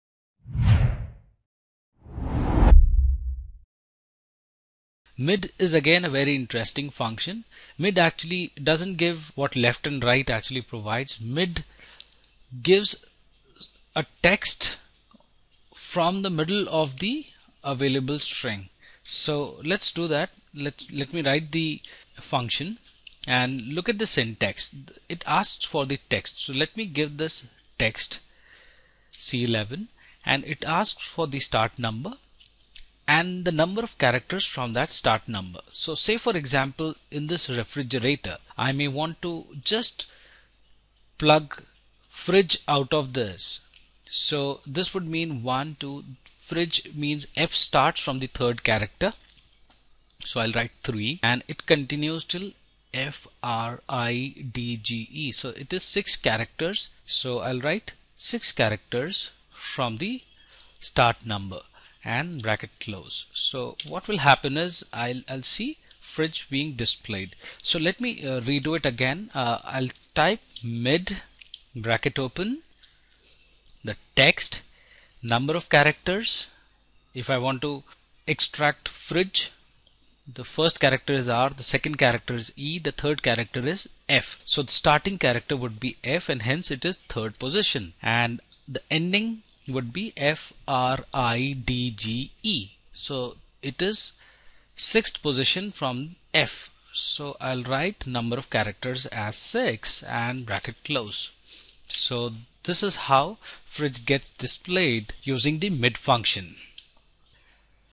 MID Function in Excel
In this video tutorial on MID Excel Function, we will see how to use the ‘MID’ Formula in excel with the help of an example.
*What is the MID Function in Excel?*
The MID function works as in what portion of the middle text we want to extract. For example, if a cell contains FIRST, then using the Mid function there, we can select the character position from which we want to extract the text and the number of characters till we want. If we want from 2 positions to the next 3 characters, then we will be getting IRS as output.
*MID Formula in Excel*
=MID(text, start_num, num_chars)
Arguments:
The MID formula has three compulsory parameters: i.e.text, start_num, num_chars.
text: From the text that you want to extract specified characters.
start_num: Starting position of the middle string.
[num_chars]: The number of characters you want to extract from the start-num.
Usually, the MID function in Excel is used alongside other text functions like SEARCH, REPLACE, LEN, FIND, LEFT, etc.

Number formatting is not part of a string and will not be extracted or counted.
This is designed to extract the characters from any side of a specified text.
If the user does not specify the last parameter, it will take 0 as default.
Num_chars must be greater than or equal to zero. If it is a negative number, it will throw the error as VALUE.
In the case of complex data sets, you need to use other text functions like SEARCH and FIND Num_chars parameters.
Use the MID function when you want to extract text from inside a text string based on location and length.
The MID function returns empty text if the start_num is greater than the length of a string.